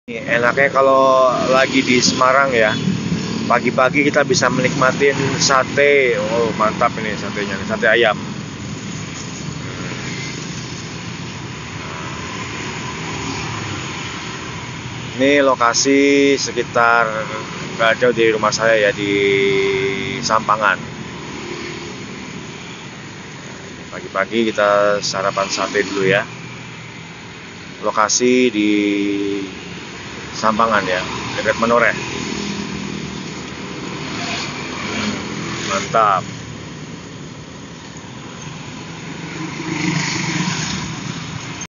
Sarapan Sate Ayam di Semarang 😂
Lokasi sekitar pasar Sampangan , mumpung lagi di Semarang 😂 Kulineeeeeeer terus kk